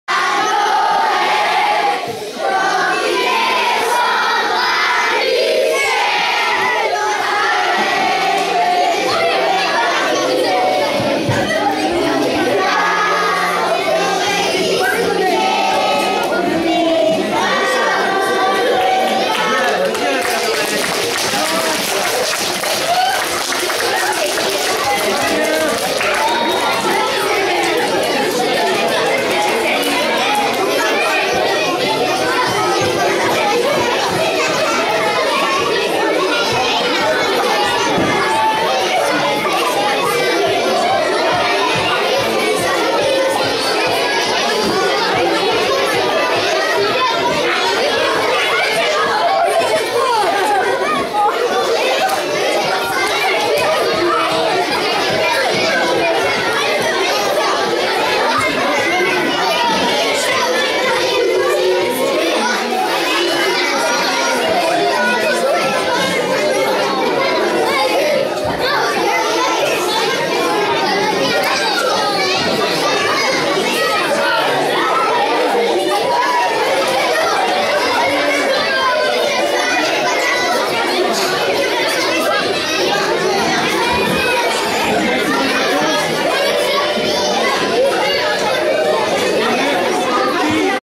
L'arrivée du père noël
Goûter de noël 2014.
École publique de Saint Aubin de Locquenay.